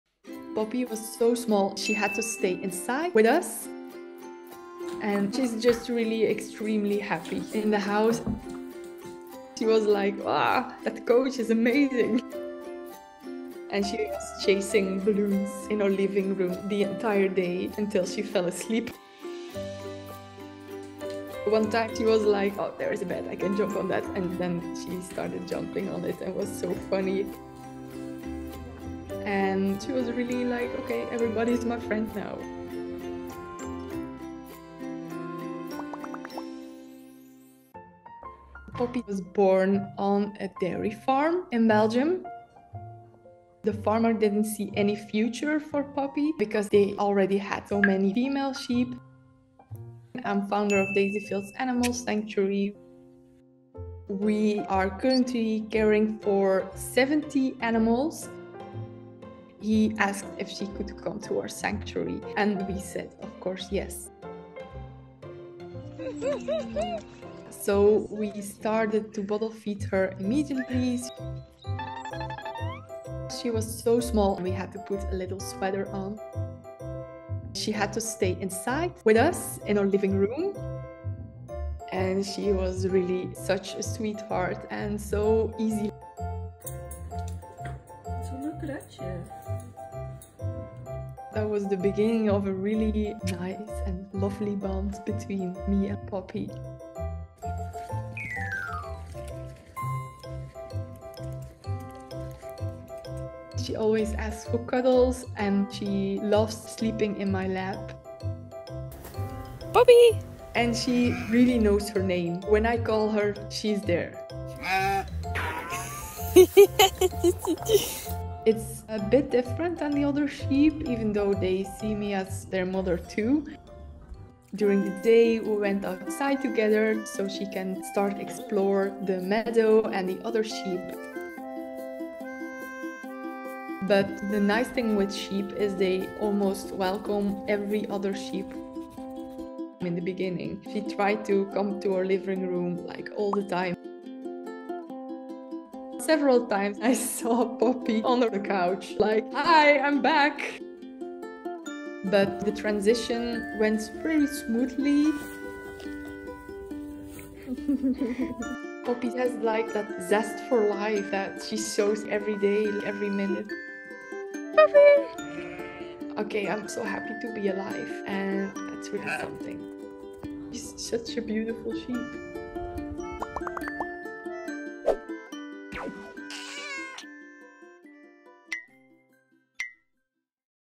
Woman Brings Sheep In Her House And Watches Her Grow | Cuddle Buddies
This little lamb is queen of the house AND a couch potato! Rescued as a little baby, Poppy wasn’t big enough to face winter so she came inside and the house became her playground. She adores her mom Evelien and even though she’s been promoted to outside, she’s still Mom’s little girl